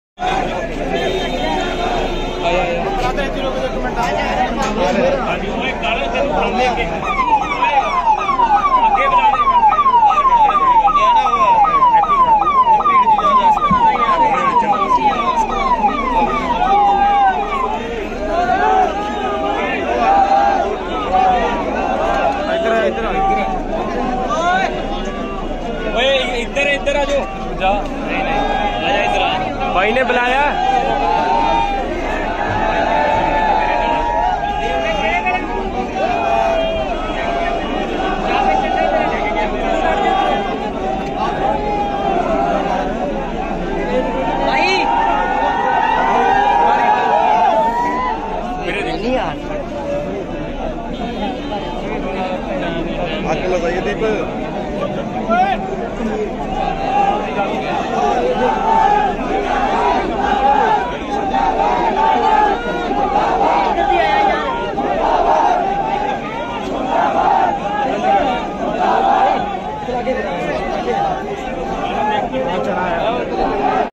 Sidhu Moose Wala के घर पर फैंस हुए बेकाबू तो फिर पुलिस ने क्या किया ?
इस वीडियो से हमारा कोई संबंध नही है । न ही हम इसकी पुष्टि करते है ।
इससे वाद विवाद होने पर bol gabru bol की कोई जवाबदेही नही है ।